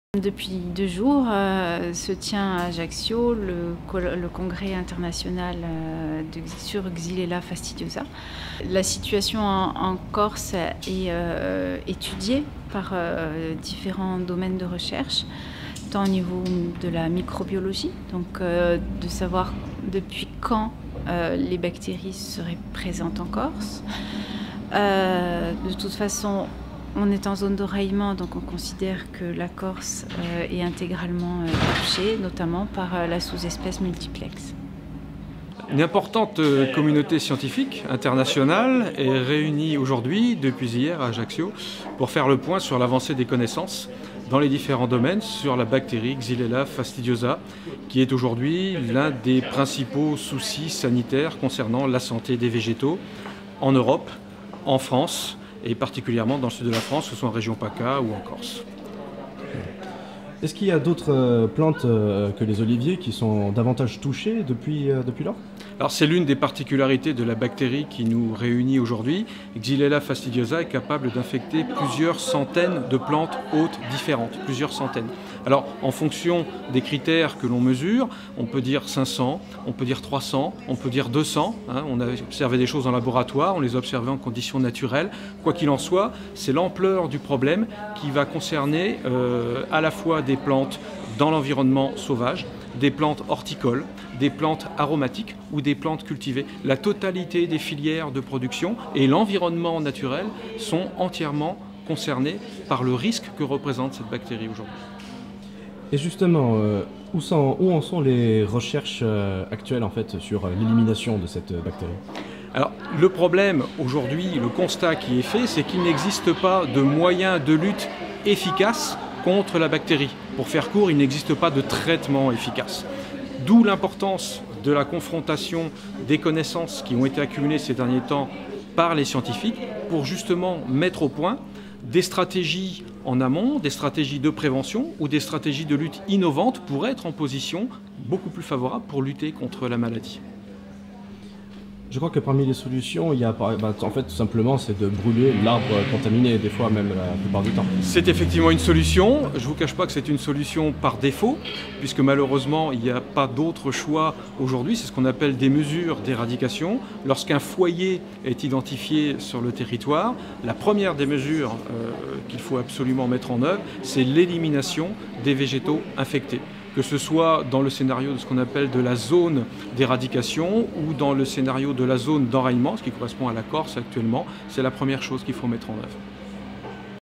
Deuxième conférence internationale sur la Xylella Fastidiosa
Du 29 au 30 octobre se tenait au palais des congrès d'Ajaccio la 2ème conférence internationale sur la Xylella Fastidiosa, la bactérie tueuse d'oliviers. Des chercheurs venus de 41 pays se sont ainsi réunis afin d'exposer leurs recherches sur la lutte contre la bactérie vorace.

Interview de Laetitia Hugot : directrice du conservatoire botanique national de Corse.
Interview de Philippe Reignault : Directeur de la santé végétale à l’Anses.